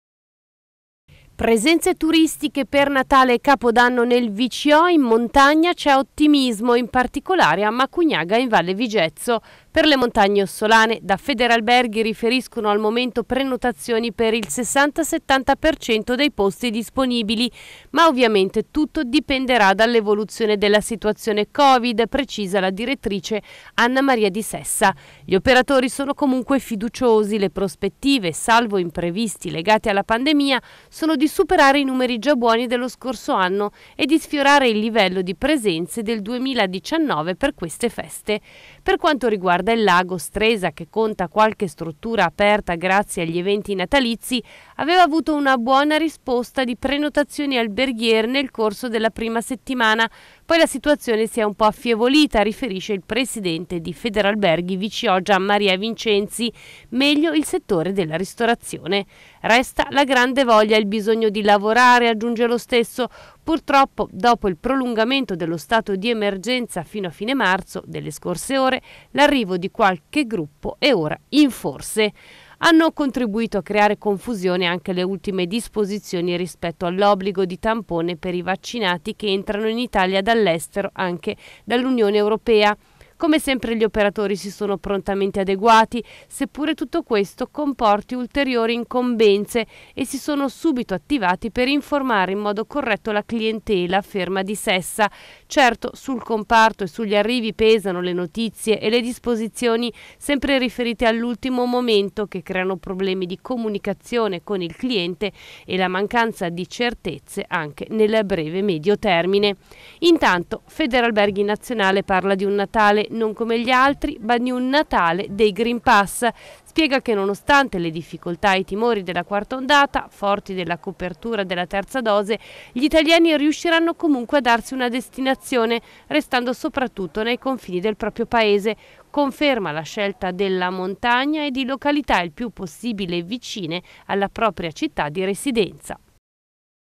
Turismo per Natale e Capodanno nel VCO: in montagna c’è ottimismo
15 dicembre 2021 - Per le montagne ossolane da Federalberghi riferiscono al momento prenotazioni per il 60/70 % dei posti disponibili. Ma ovviamente tutto dipenderà dall’evoluzione della situazione Covid